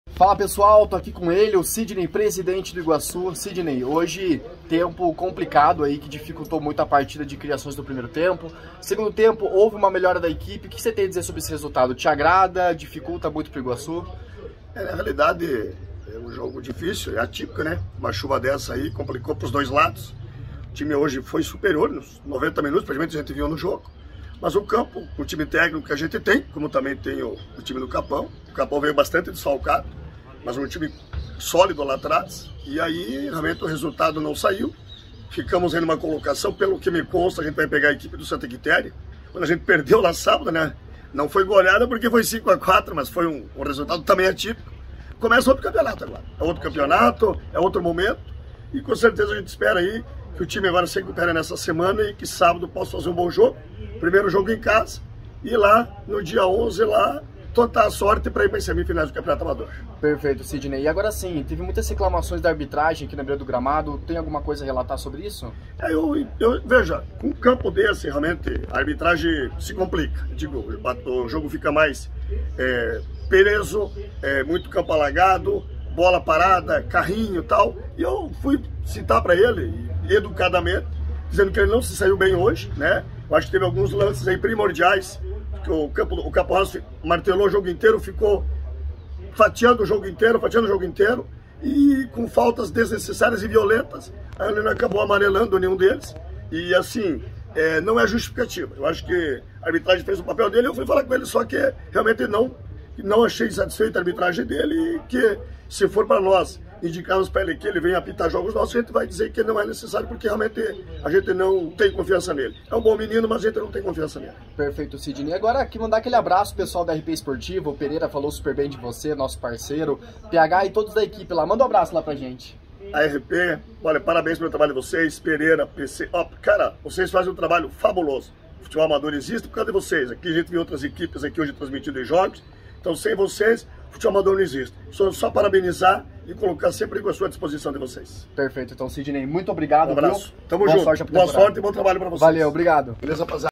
Entrevista com o Sidney
Entrevista com o Sidney,  Presidente do SOBE Iguaçu  pós jogo Iguaçu 0x0 Capão Raso (Reportagem André Ciampi)